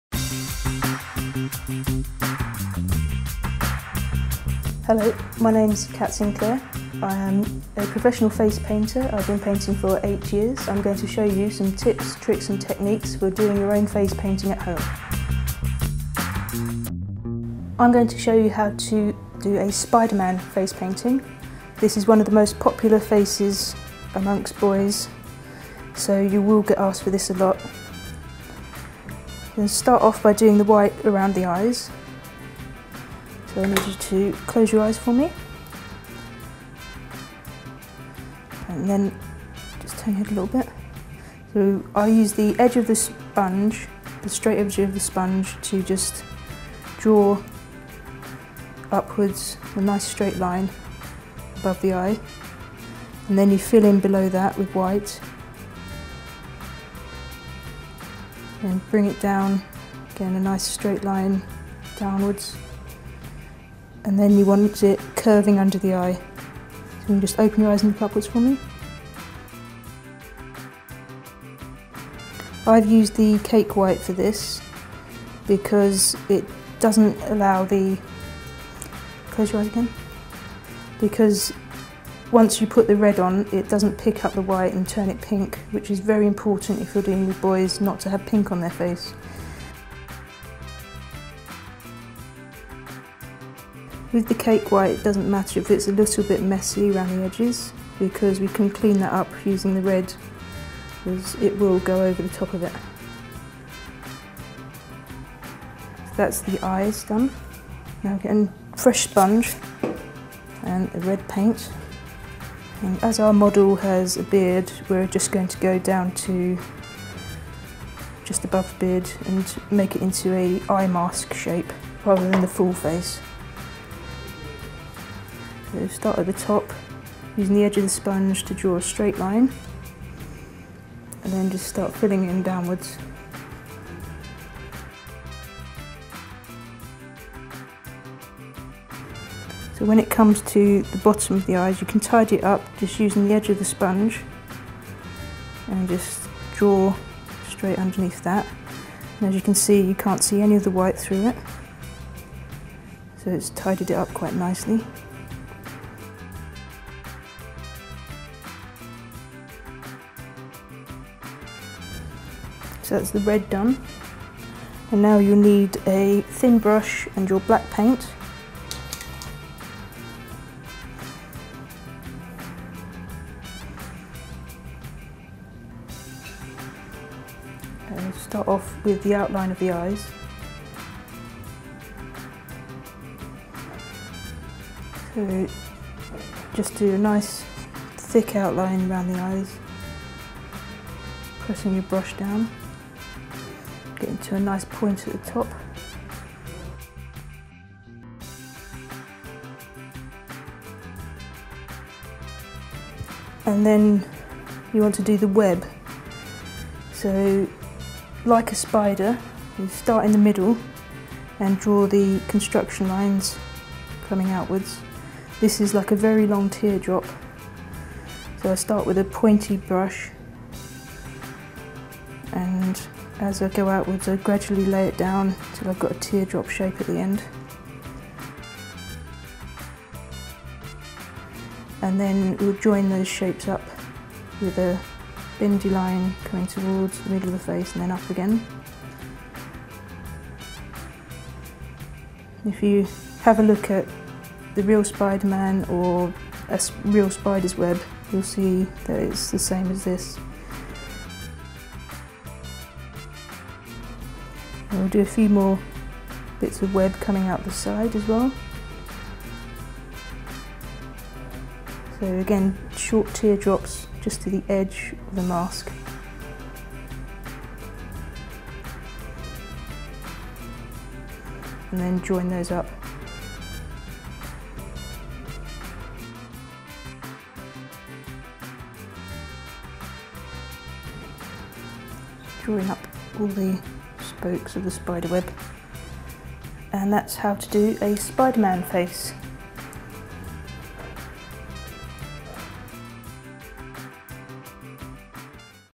How To Have A Spiderman Facepainting
Watch How To Have A Spiderman Facepainting from the pioneers of how to videos. This tutorial will give you informative instructions to ensure you get good at face painting.